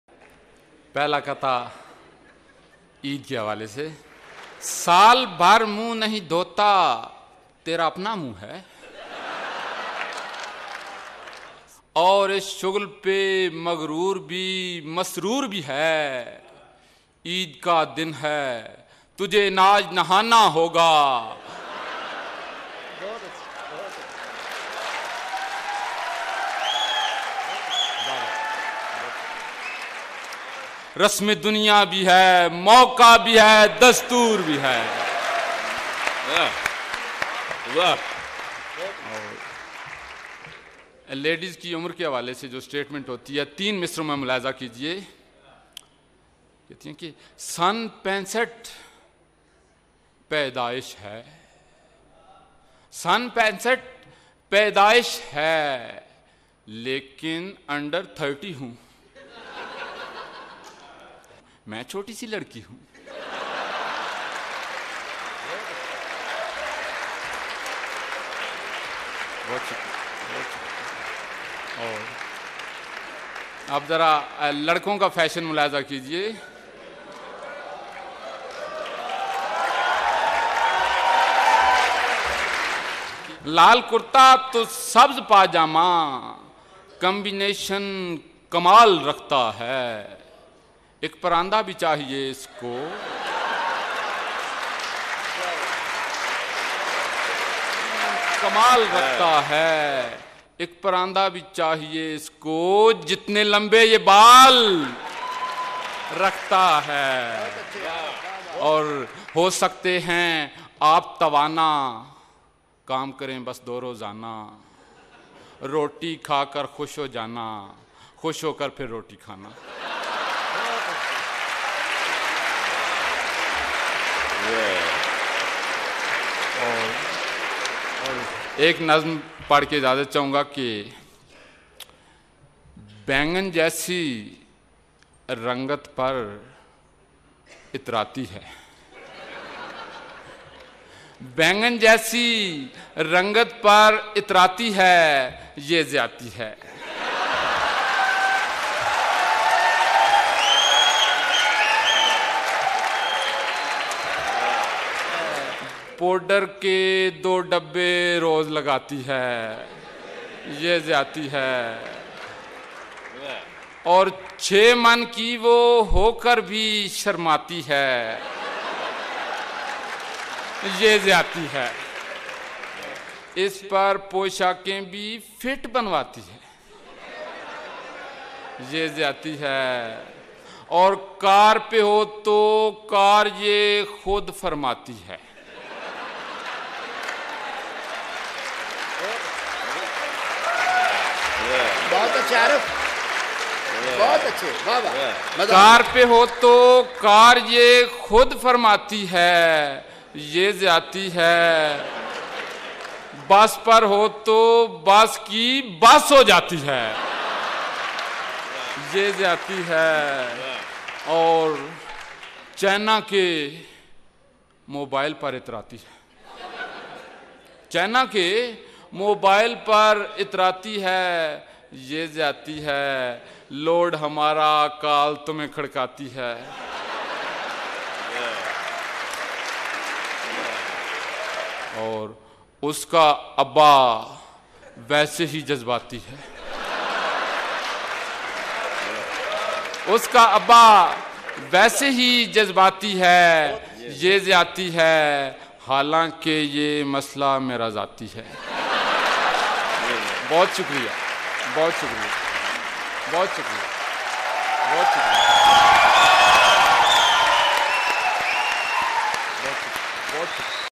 Mohammad Arif's Mazahiya Urdu Shairiمحمد عارف کی مزاحیہ اردو شاعرئ ۔
Mohammad Arif's funny Urdu poetry - محمد عارف کی مزاحیہ اردو شاعرئ
Mohammad Arif is a well know Urdu poet and has participated in a lot of literary session and grand mushairas. His specialty is writing funny Urdu poetry for which he is liked and encouraged by the audience everywhere.

محمد عارف اردو کے ایک جانے پہچانے شاعر ہیں اور انہوں نے بہت سارے ادبی محفلوں اور عظیم مشاعروں میں حصہ لیا ہے۔ ان کی خصوصیت مزحیہ اردو شاعری لکھنا ہے جس کے لئے انہیں ہر جگہ سامعین نے پسند کیا اور حوصلہ افزائی کی۔